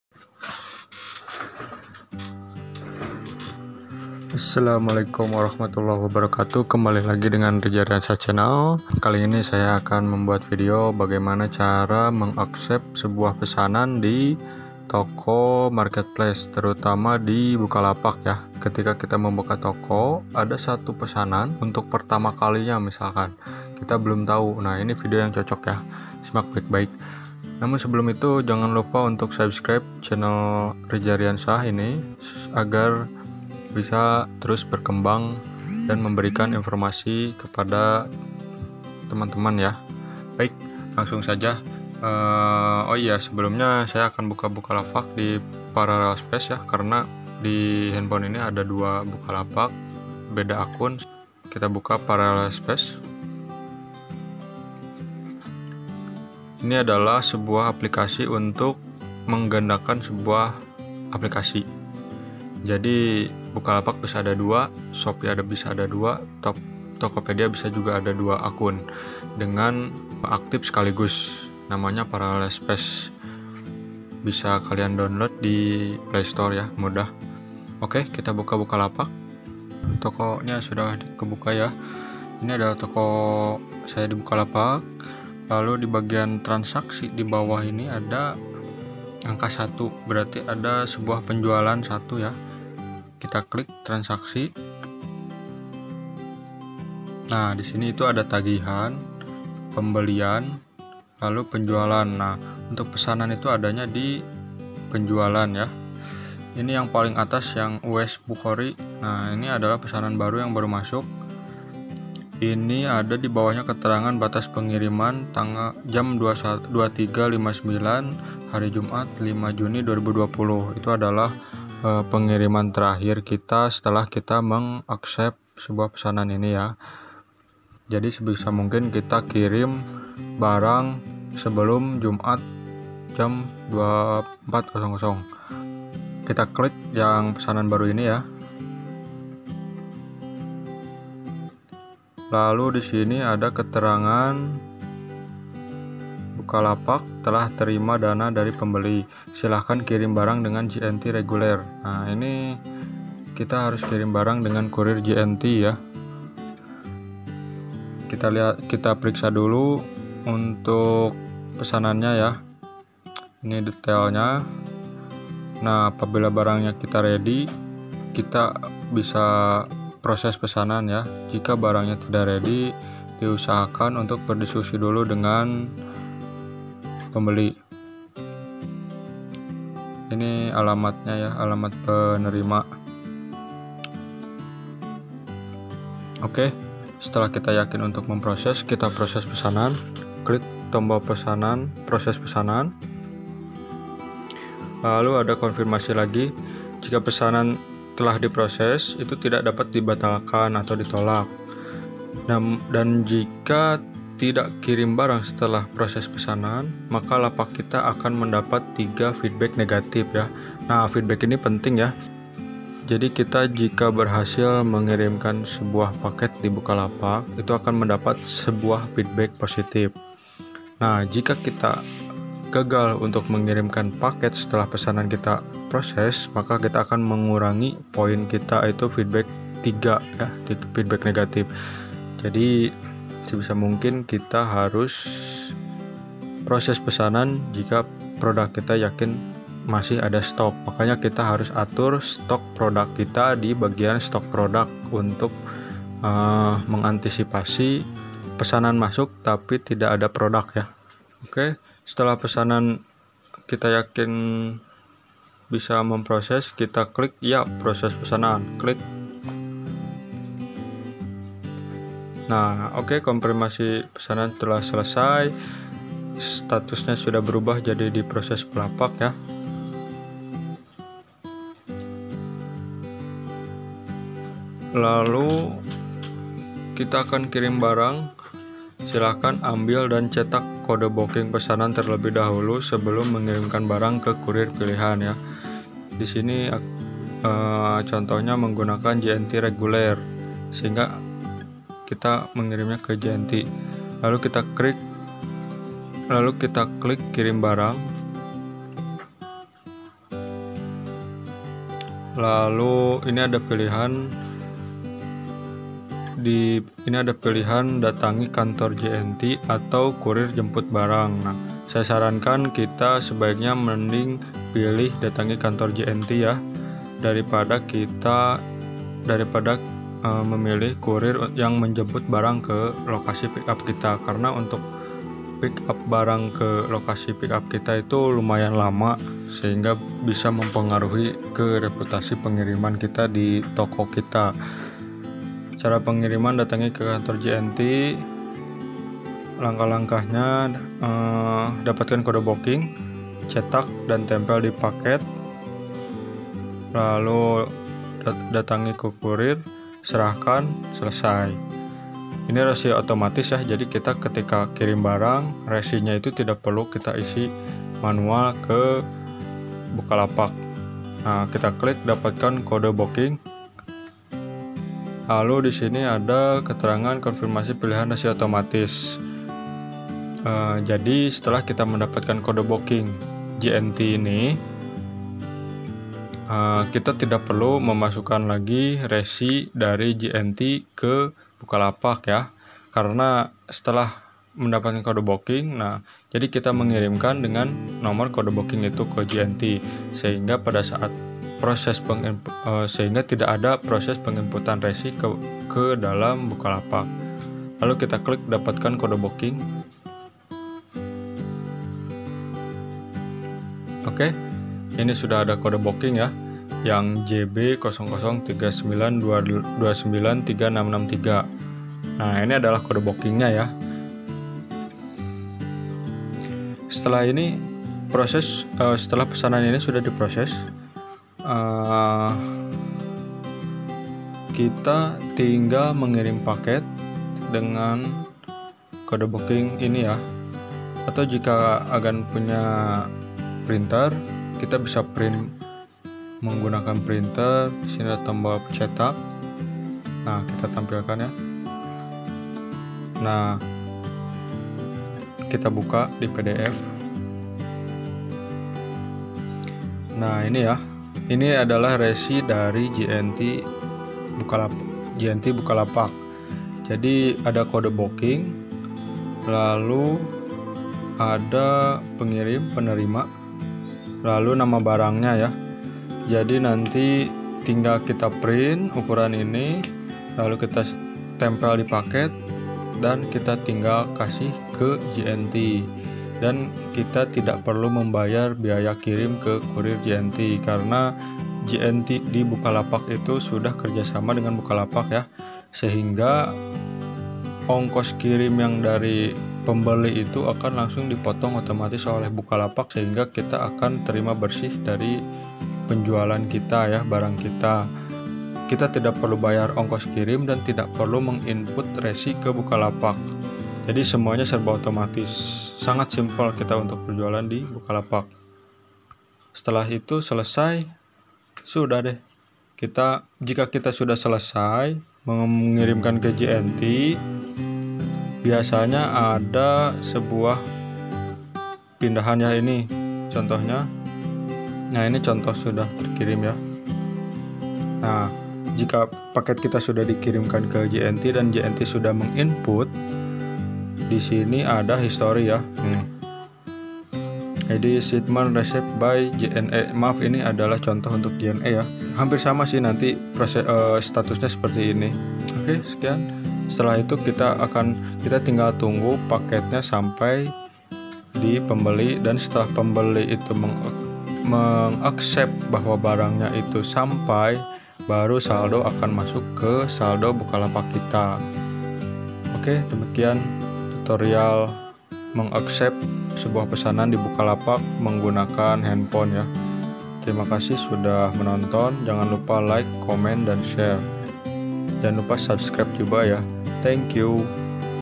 Cara Terima Pesanan masuk Bukalapak dari Hanphone Via J&T
Hallo,,

Kali ini daya berbagi cara untuk menerima pesanan di Bukalapak di accept menggunakan Hp bagi yang tidak punya PC atau Laptop, pengiriman menggunakan J&T..

Terima kasih,
Semoga Bermanfaat,
Salam, Rizal..